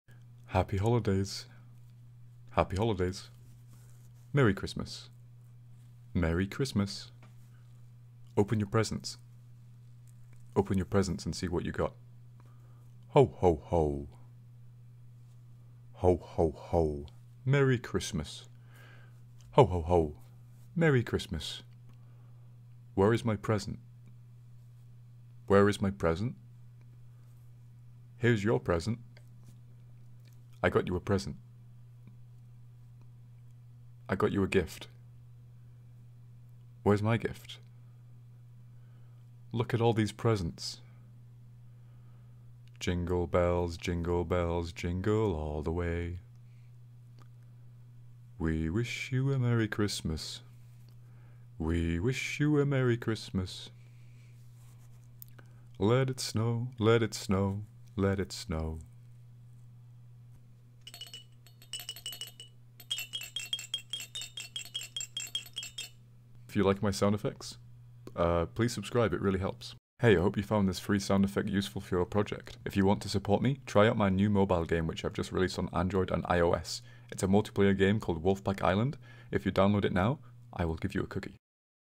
Christmas Sound Effects
Various sound effects you can use for any project, whether it's christmas-themed or not. You don't need to seek my permission to use my sound effects.